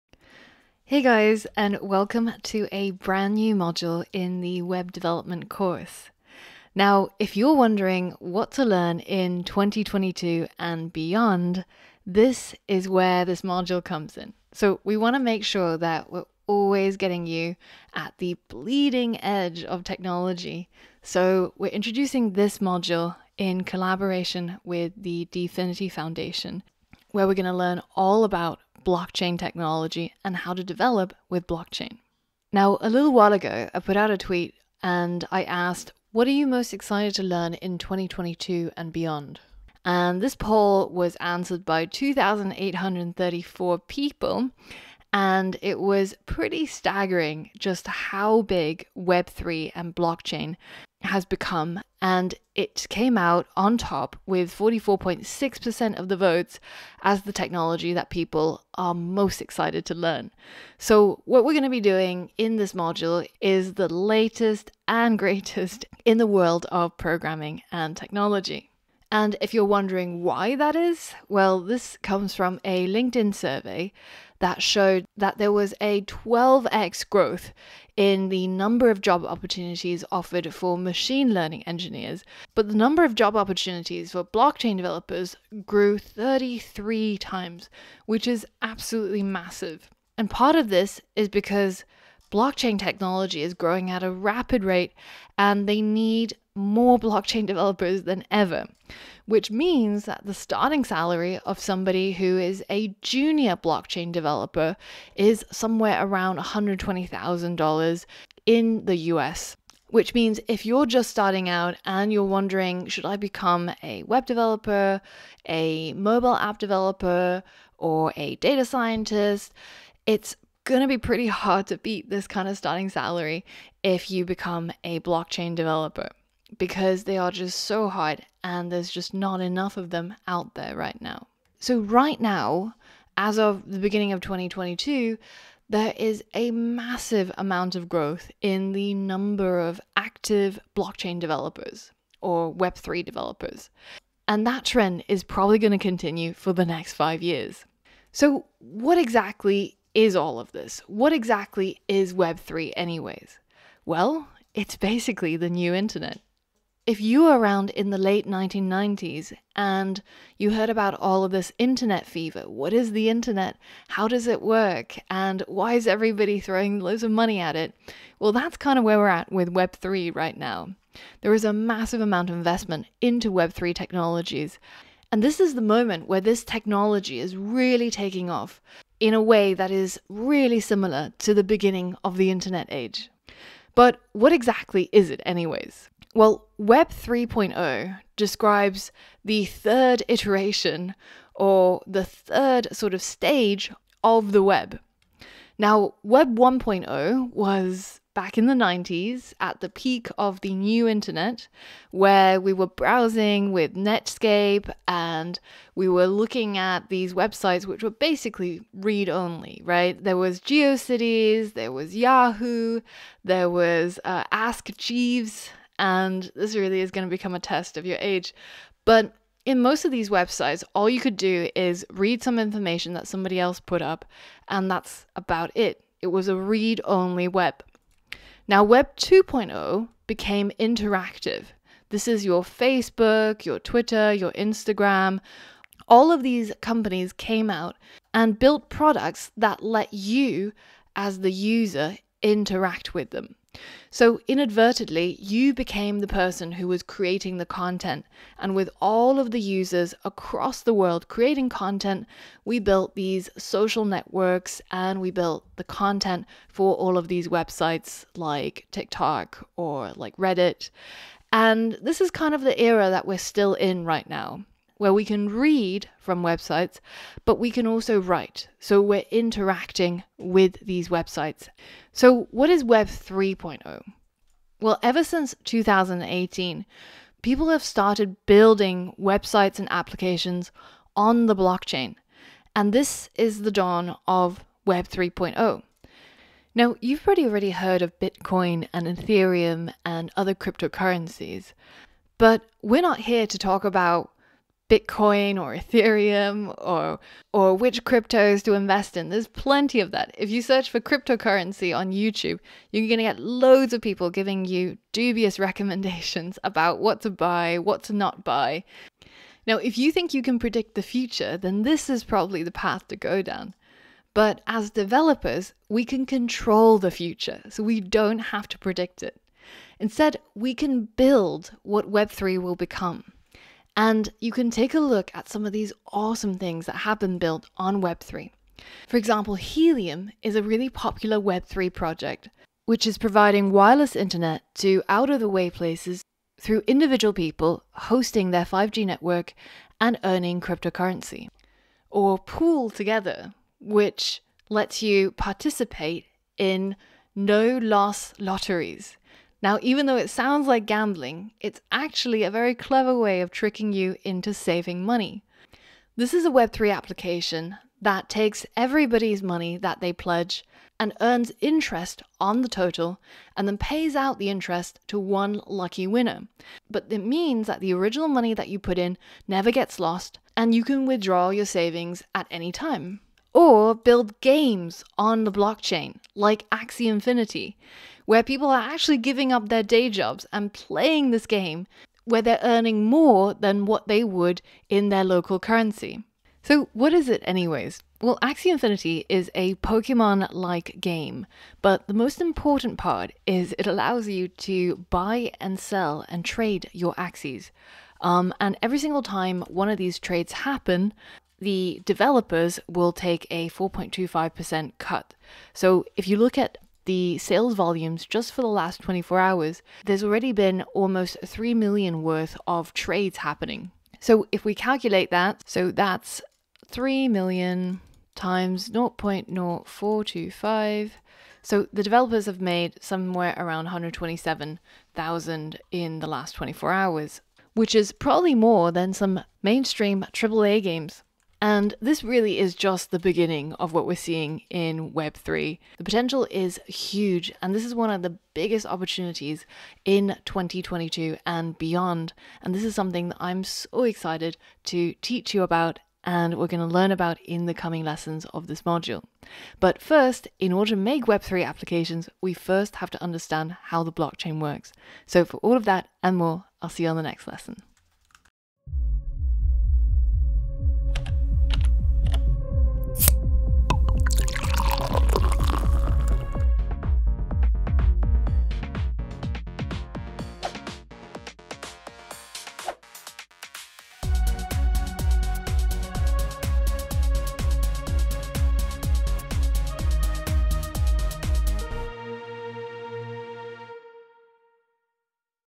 What is Web3?
This is a preview of our brand new 10-hour module on Web3 and Blockchain development that just got added to our best-selling The Complete 2022 Web Development Bootcamp.